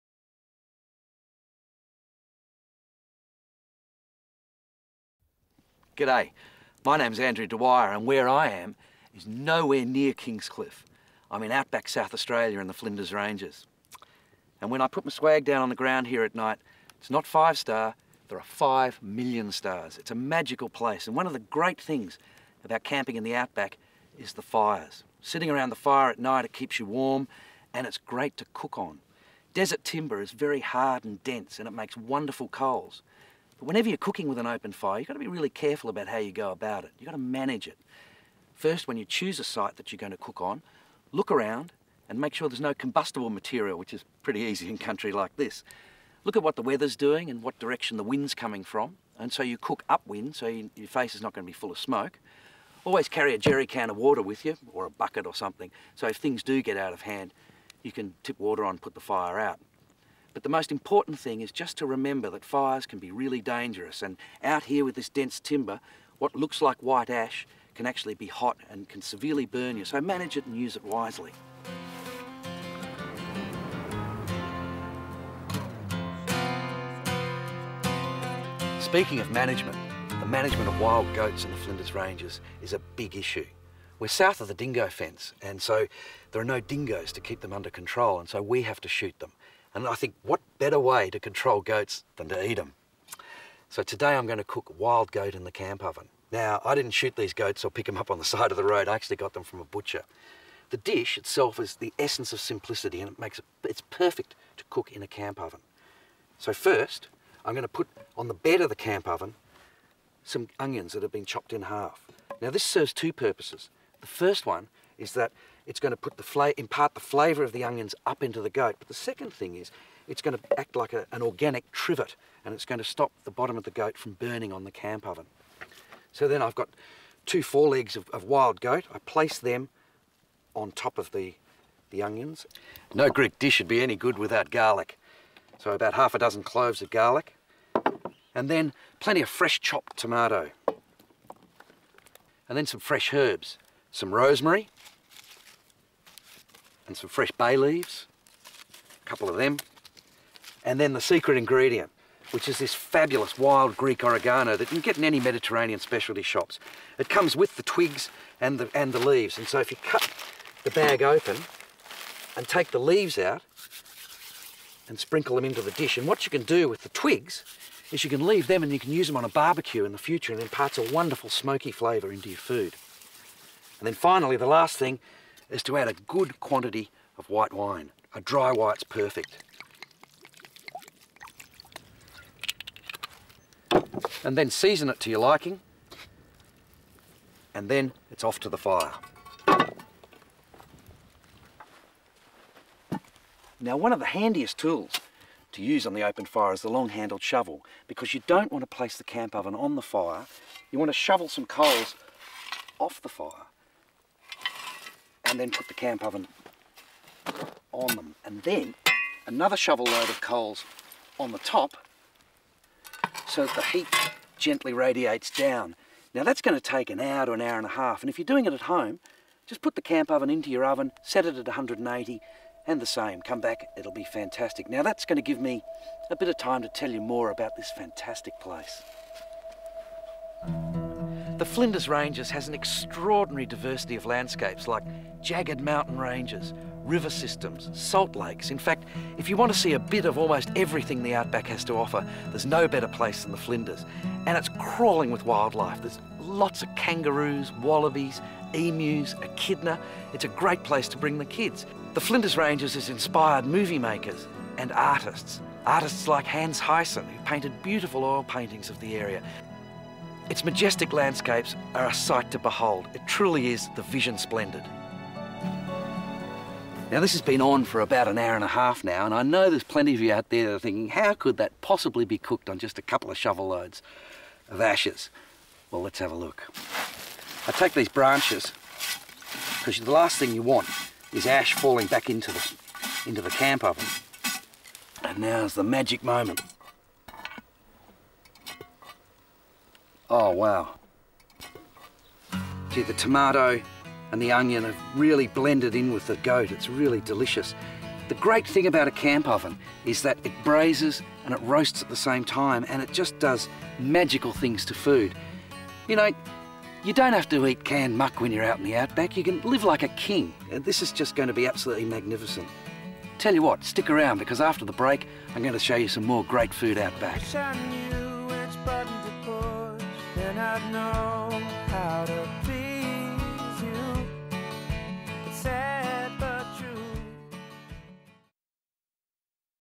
wild goat
Andrew cooks delicious legs of wild goat in a Dutch oven with tomatoes, white wine, wild greek oregano and garlic. Mouth watering and delicious. Fire Cooking at its best